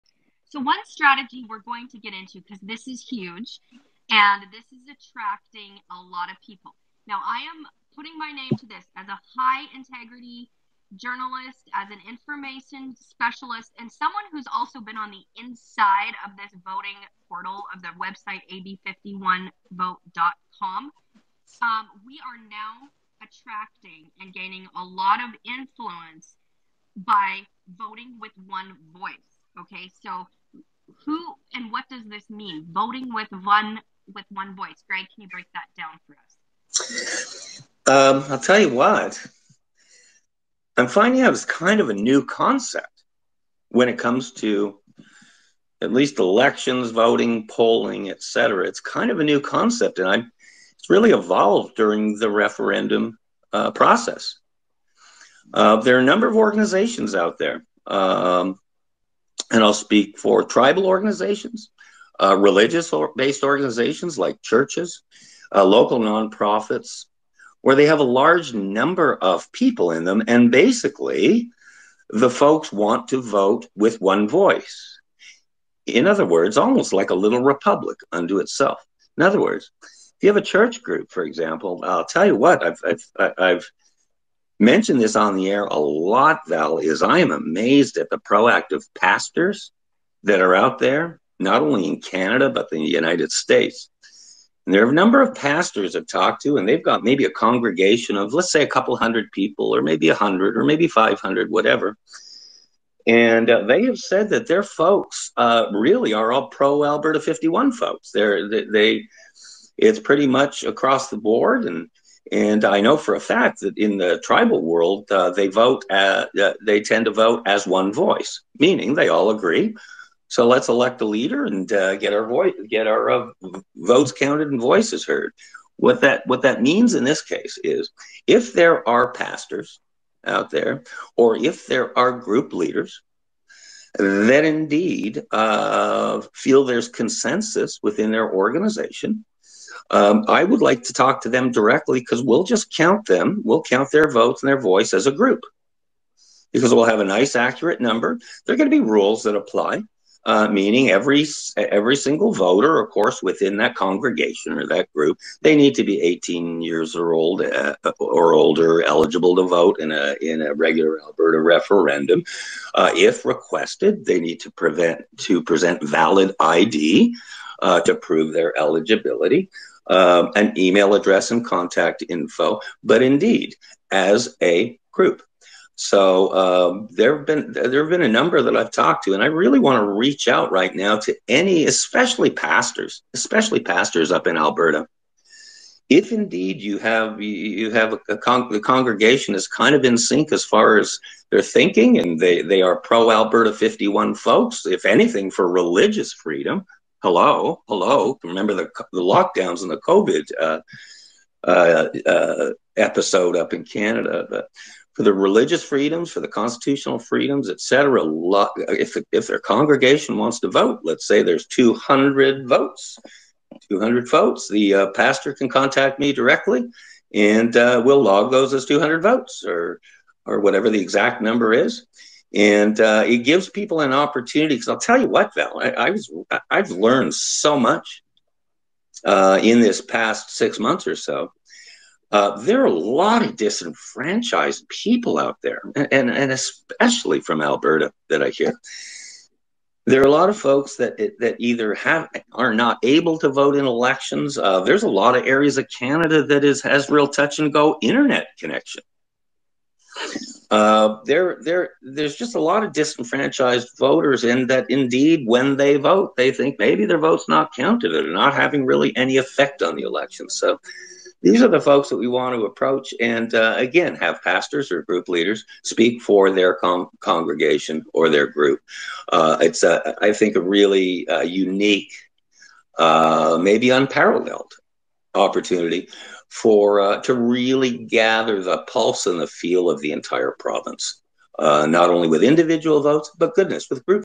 MEAKIN: Groups Can Vote With One Voice!
Groups are choosing to vote with One Voice. Val Olson hosts the AB51PODCAST with guest Greg Meakin, GregMeakin.com.
* Val's show is featured on Twitter Spaces @HomeMediaMom.

   AB51VOTE.COM
   AB51PROJECT.COM
   AB51PODCAST.COM